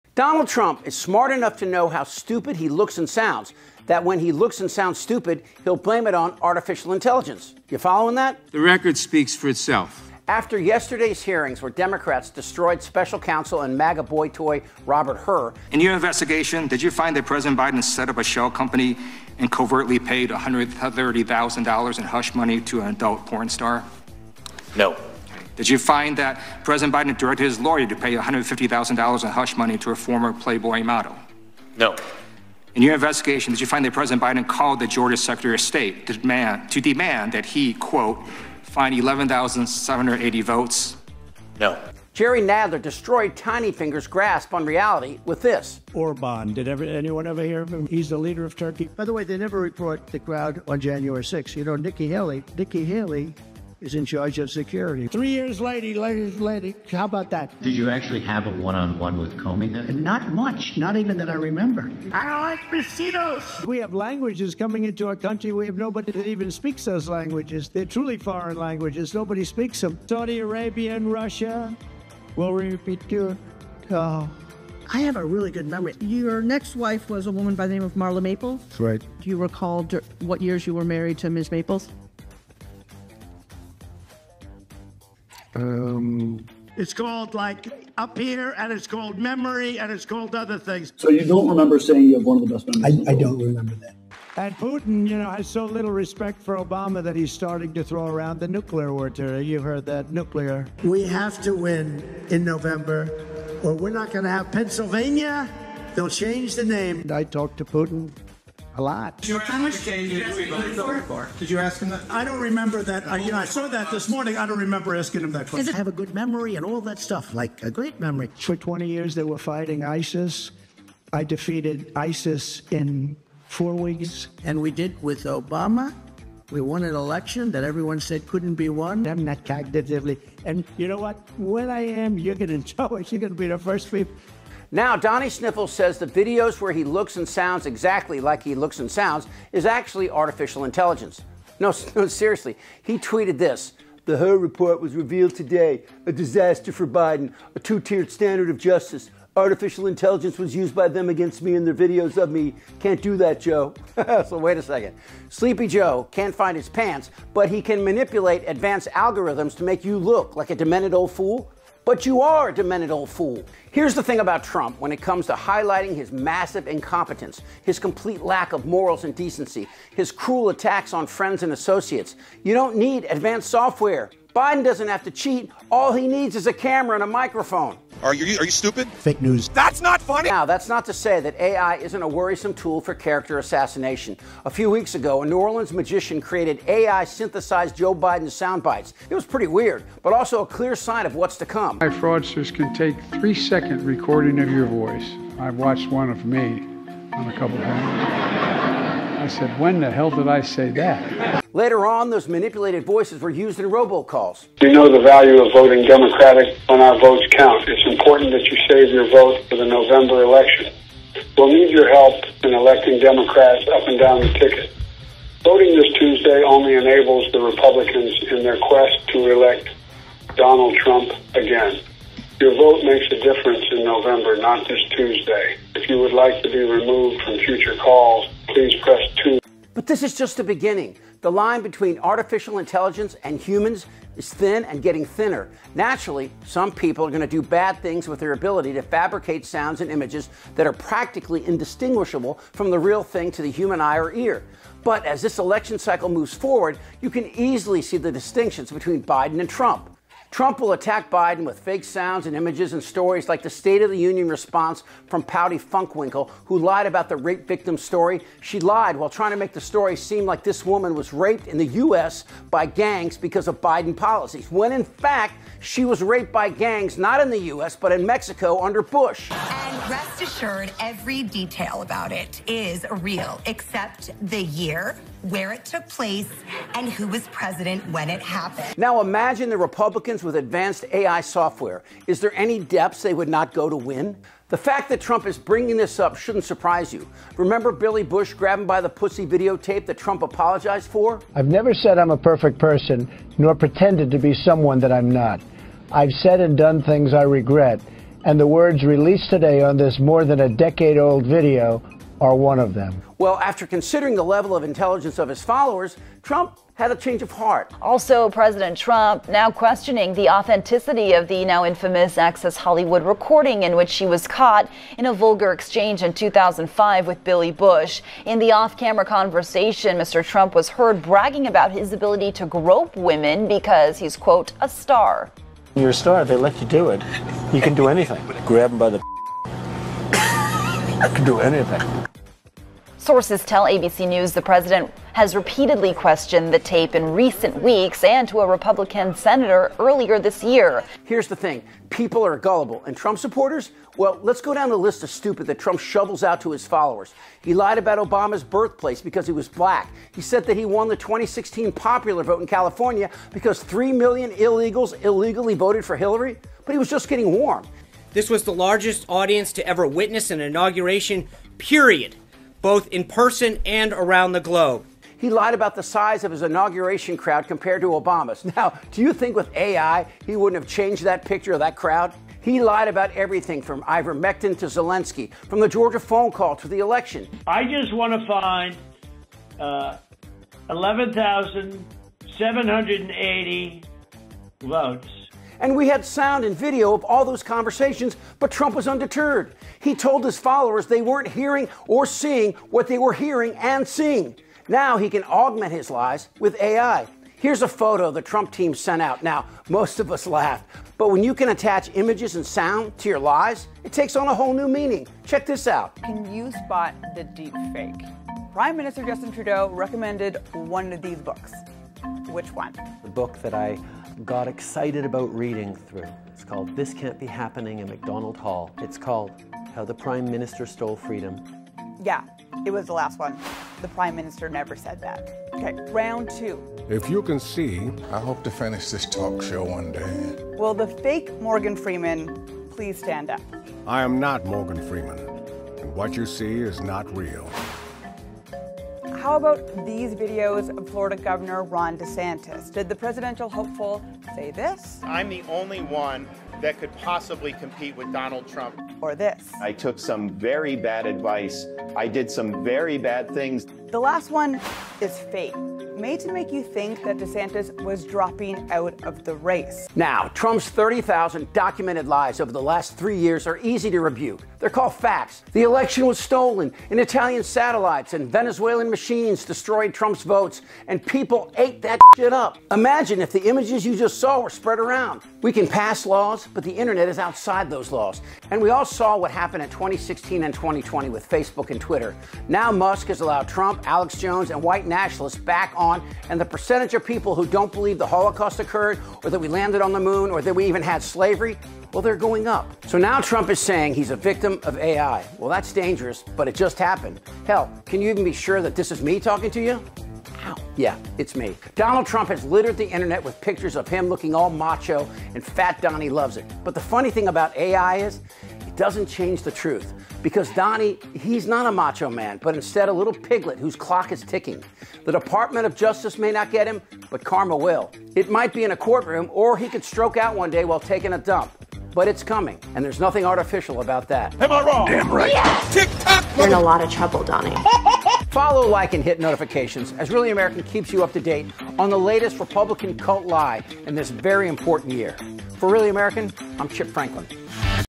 Trump RAGES With Most BIZARRE LIE Yet To Hide His Mental Decline
After a House Congressional Committee detailed the former presidents cognitive decline, the former president took to truth social to claim the videos use Artificial Intelligence to showcase his decline. Really American host Chip Franklin breaks it down.